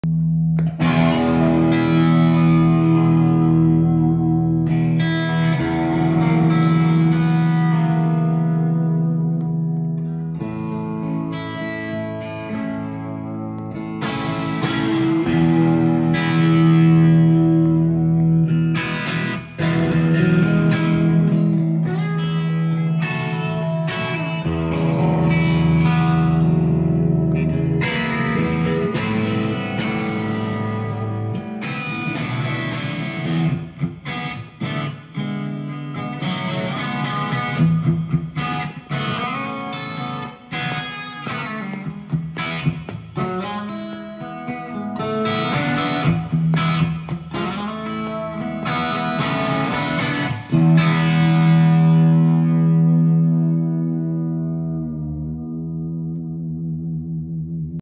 Kauffmann ’63 T B16
Baas Kauffmann always builds guitars during his holidays. For fun and for himself. This machine features a weight-relieved alder body with a zebrawood top. The entire body is built around the Bigsby B16 for perfect tuning stability and sound. Pickup is a Kauffmann XL T. Neck is a period correct C shape with 6230 frets and 7.25" radius, Gotoh vintage tuners